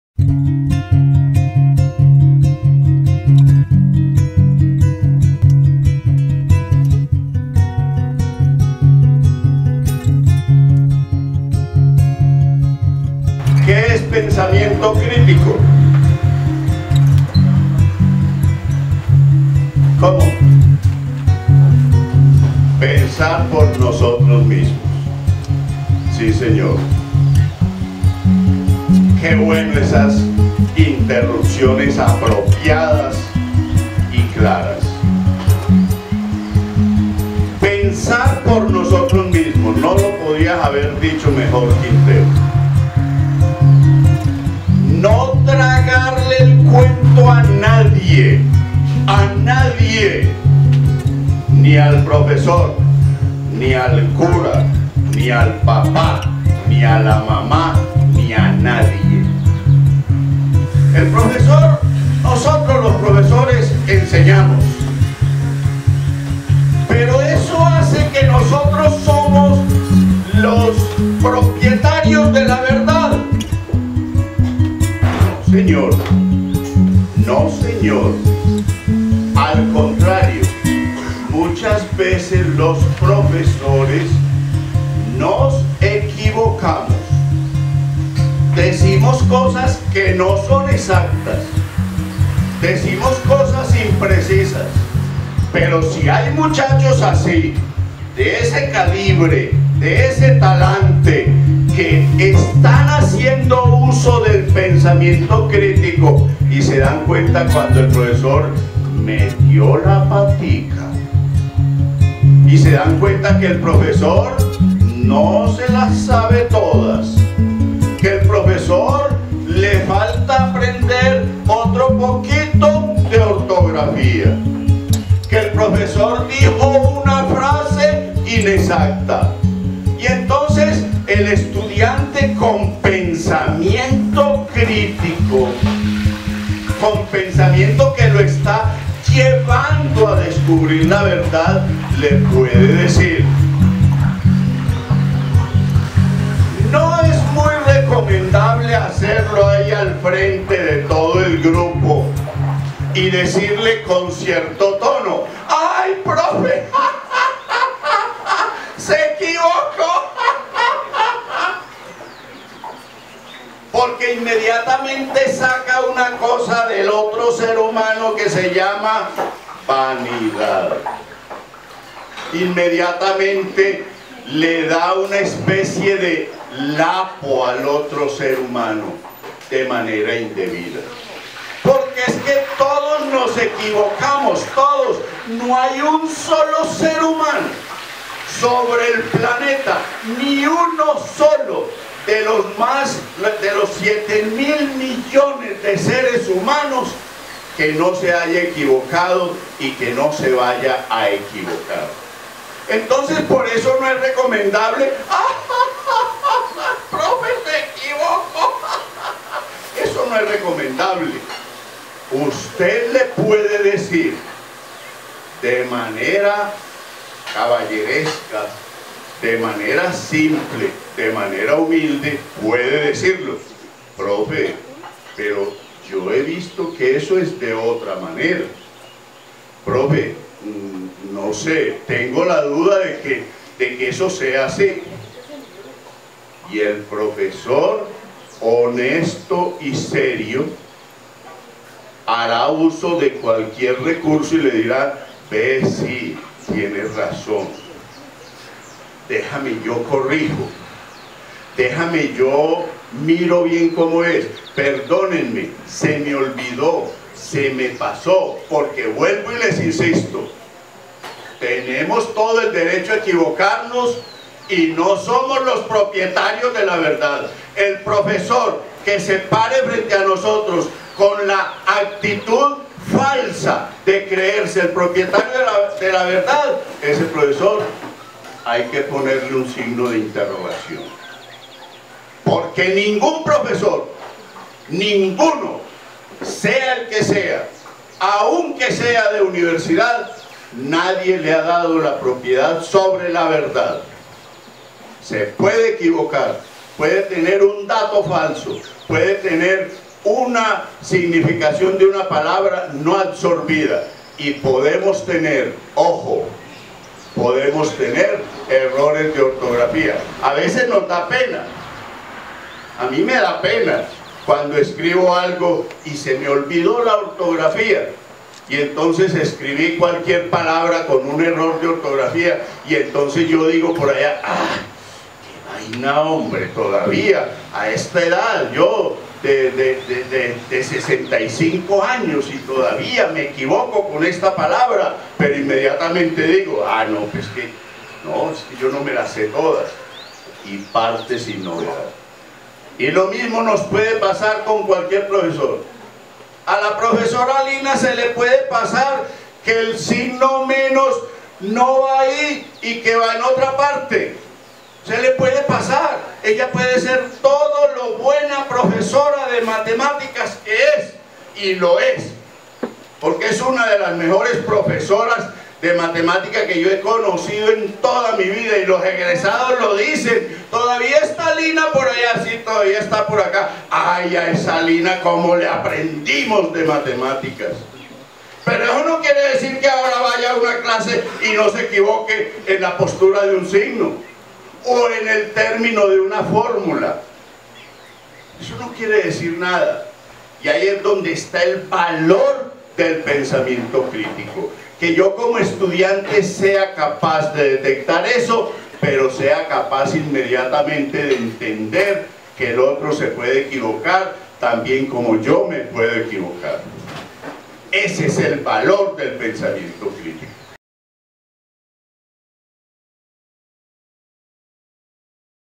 Espíritu Anglo Español Pensamiento crítico
En el taller 12 y 13 del EspírituAngloEspañol Analizamos los puntos mas importantes del pensamiento crítico, de la cuarta (4ta) revolución industrial y la inteligencia artificial.

Rector Rubén Darío Orozco.